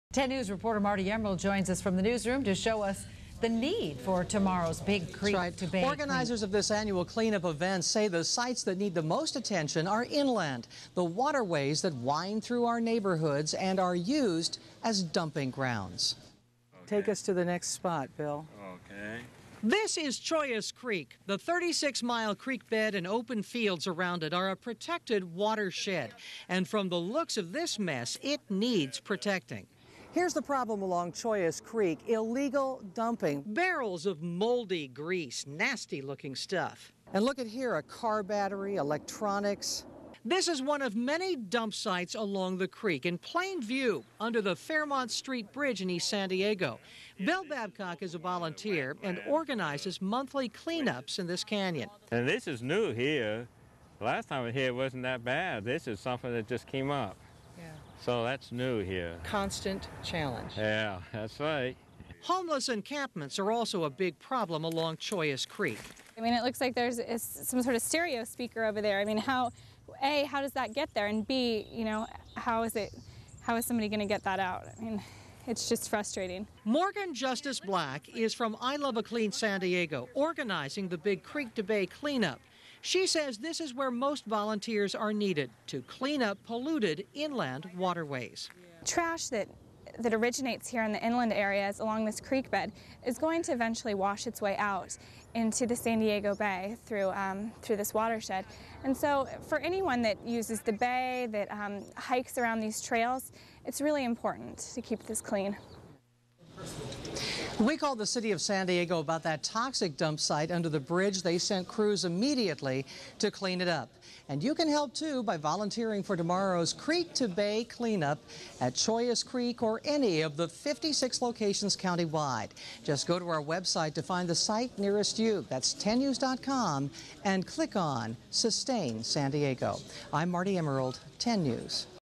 Trash at Chollas Creek
This news video is from KGTV news with Marti Emerald from San Diego, CA. This is the story taken on April 27th 2007 just the day before 5th Annual Creek to Bay Cleanup. Bill Babcock and Morgan from I Love a San Diego did the story is about trash from illegal dumping and from transients. This is at Chollas Creek near 47th Street and Fairmount Avenue where there have been an illegal dumping and trash that's been around for a long time. I am also the leader of Friends of Chollas Creek 47th Street.

Friends of Chollas Creek 47th St.